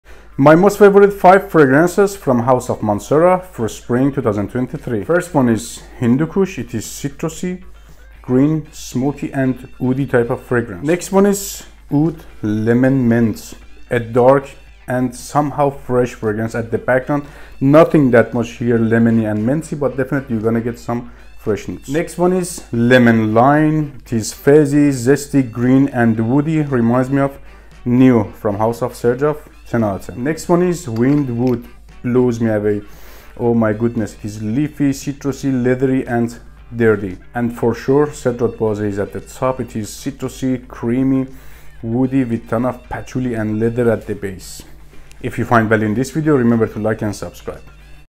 Top 5 Best Mancera Fragrances for Spring 2023 | Fragrance Cologne Perfume
My favourite 5 fragrances colognes from house of Mancera for spring 2023
Mancera Wind wood
Mancera Hindukush
Mancera Lemon Line
Mancera Wind Wood
And Mancera Cedrat Boise!